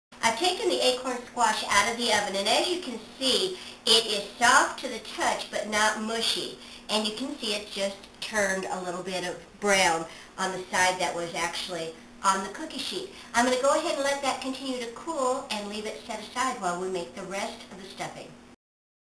Real Texas Wild Rice Stuffed Acorn Squash Part 3 of 5
Ramona Werst shares her recipe for Real Texas Wild Rice Stuffed Acorn Squash.  You can download and print a full version of the recipe from the blog.